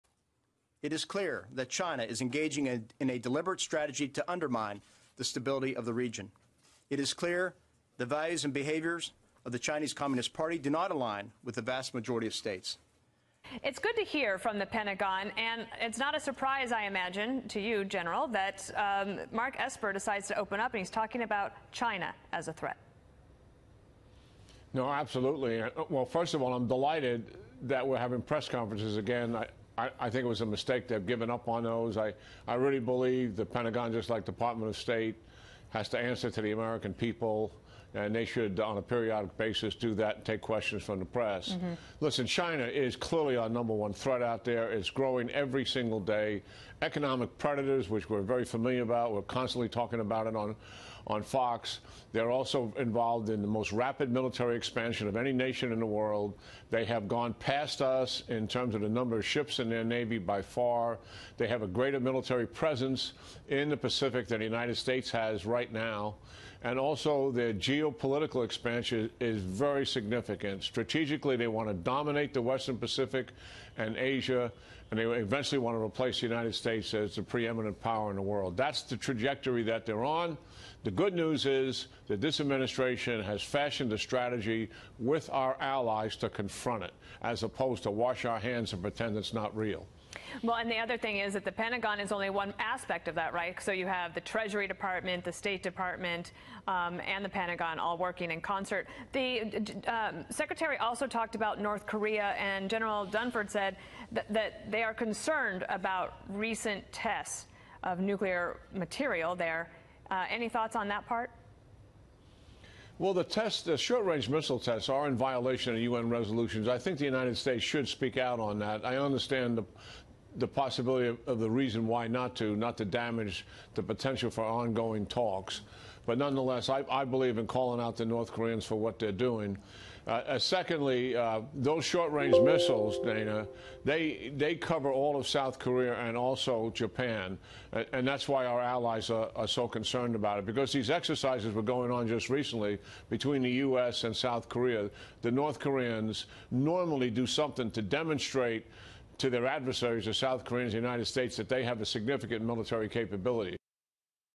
戰友之聲2019/8/29號：美新国防部长在28日点名“中共是亚太地区稳定坏破者”；基恩将军更直接说“中共显然是美国头号威胁”
如果您有新兴趣或能力为视频加上字幕,将不胜感激！！！各国语言都可以，谢谢！